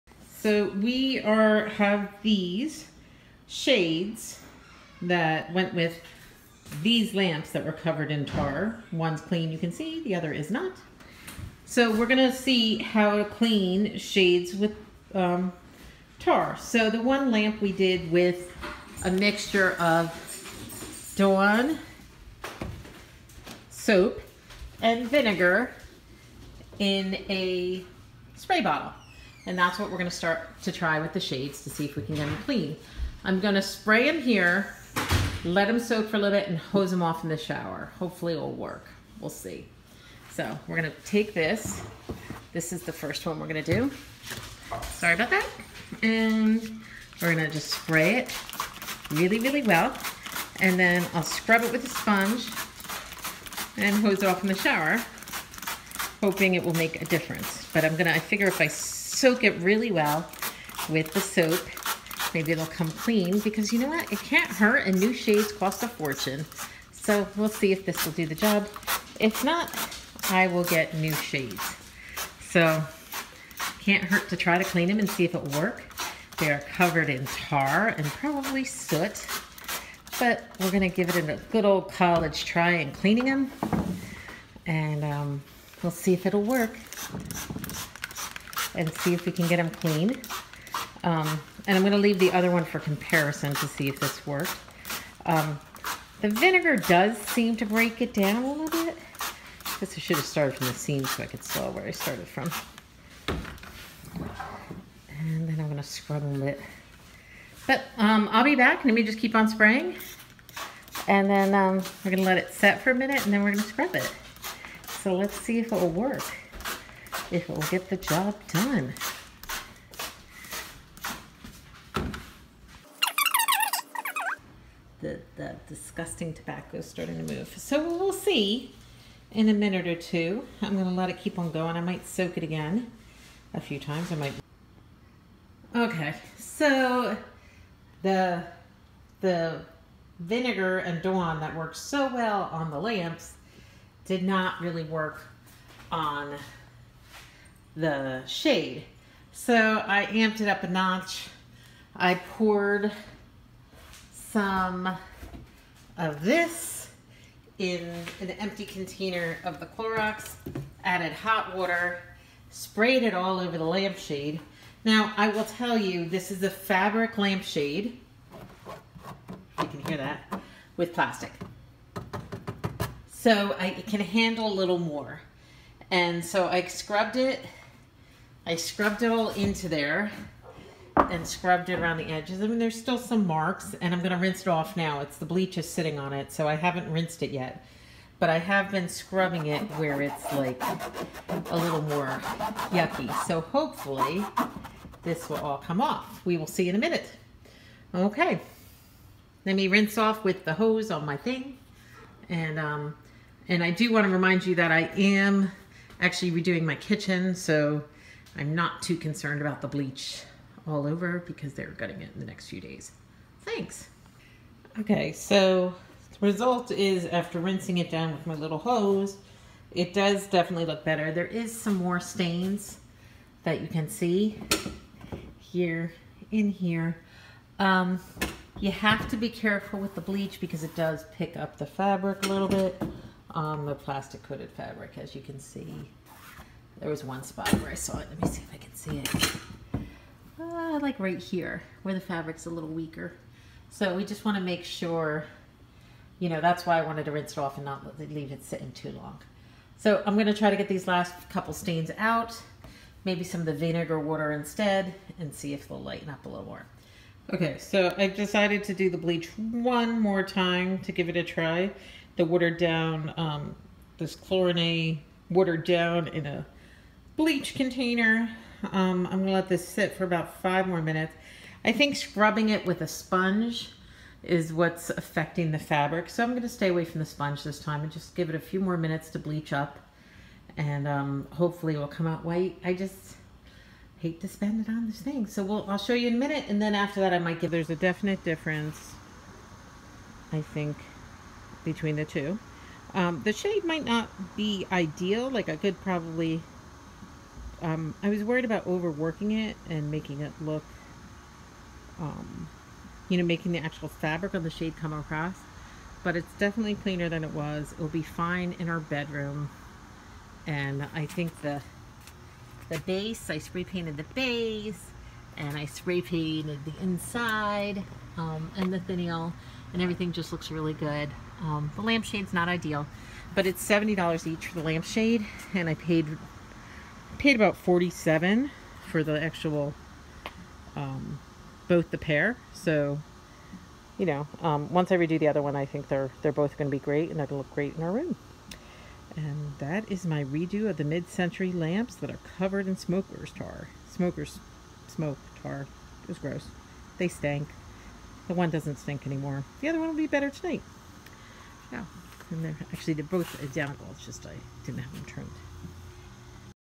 How to remove Tar from a lampshade diy smokers damage mid Century repair
This is how to remove smokers tar damage from lampshades repair recycle.
Lamp shades are expensive and larger mid Century ones are mot made.
This is how I removed it
Please note this is plastic based shade not paper covered in fabric.